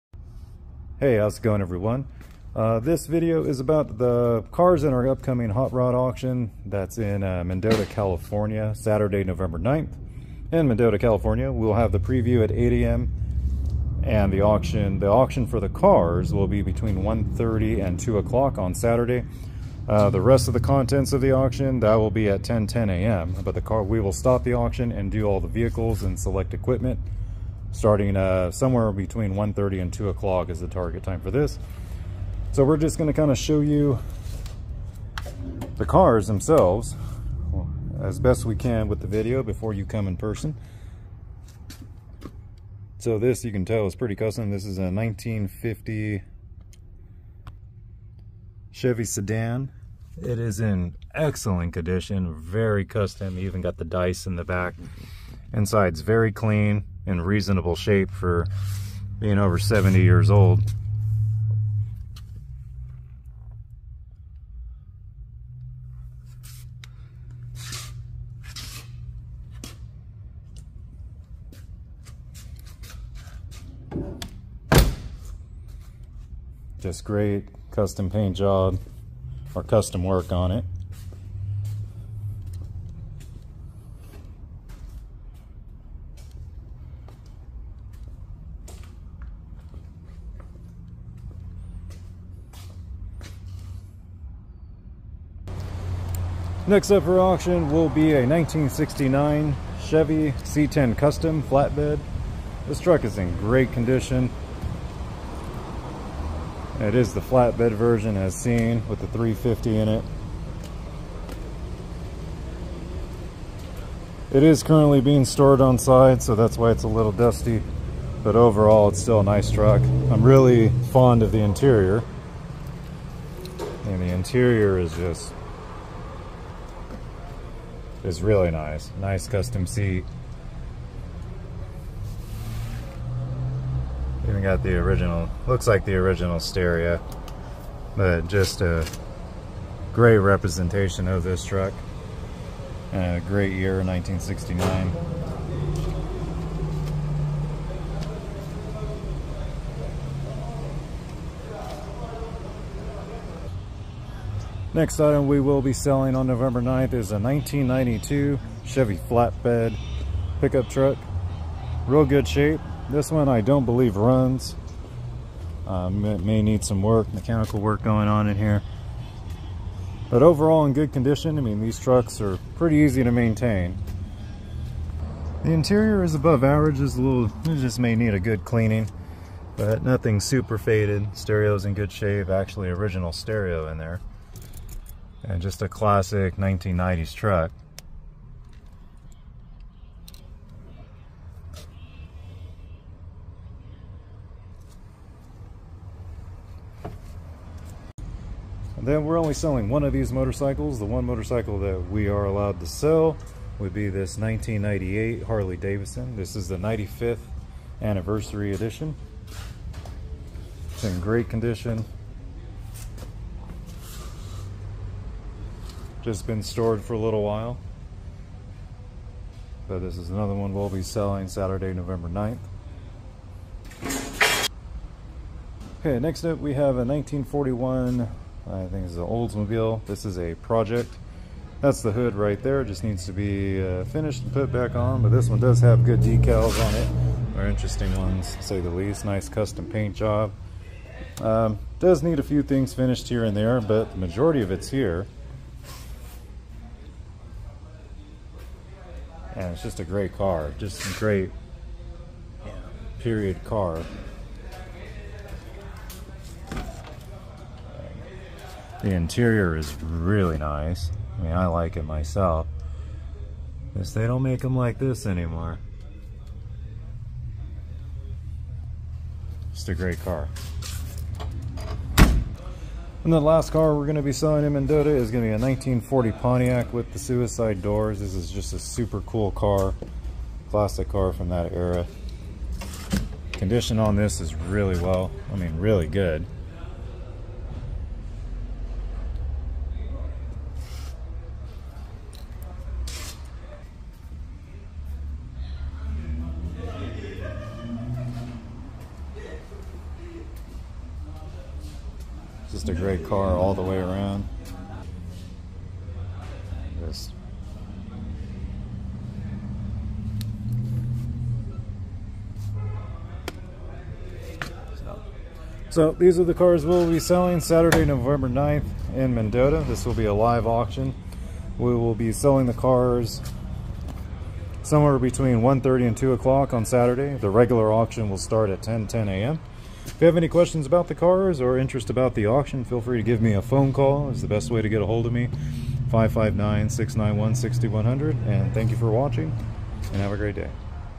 Vehicle Preview Video for the EPIC Hot Rod LIVE AUCTION in Mendota. Saturday Nov. 9th 10am
Featured in this video are the cars and trucks to be sold during our live auction in Mendota, Ca

These will be offered for auction on Saturday between 1:30pm to 2:00pm